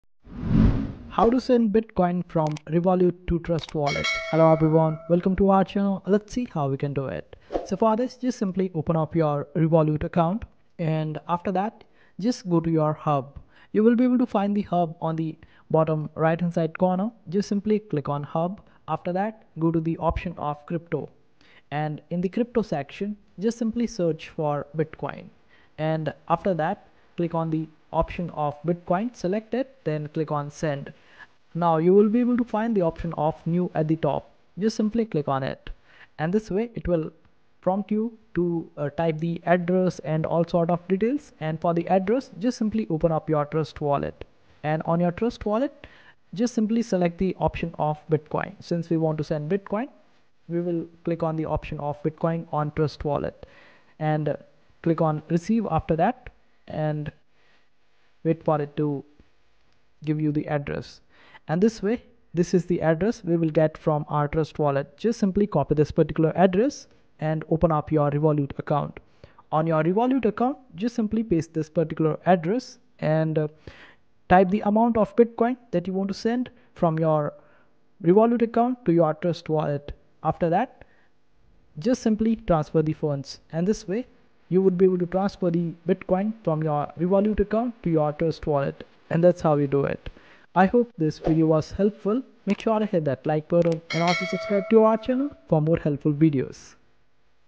How To Transfer Bitcoin From Revolut To Trust Wallet (2025)
How To Send Bitcoin From Revolut To Trust Wallet (2025) Fast Tutorial